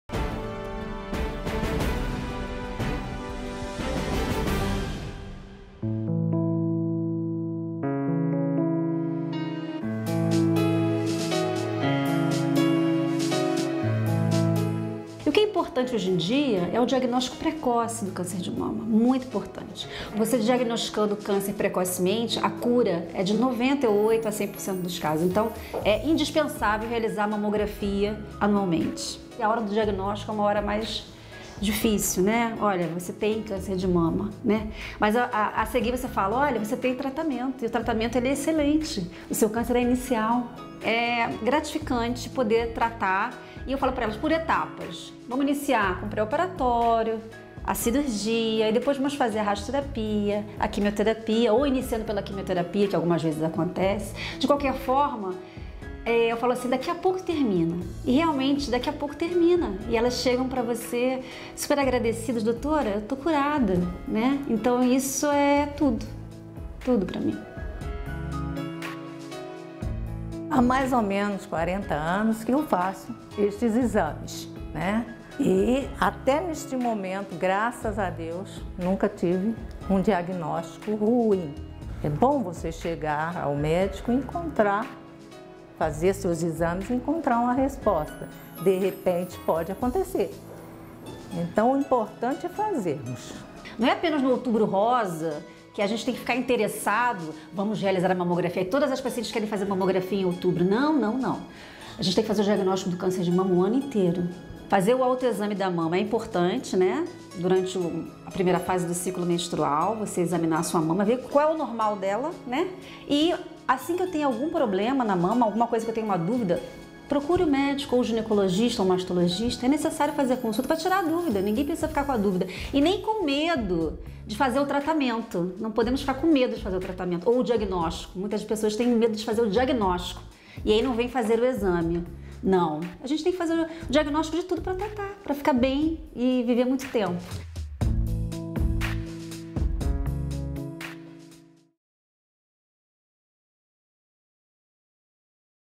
Campanha de combate ao câncer de mama
Ministério da Defesa realiza campanha de combate ao câncer de mama.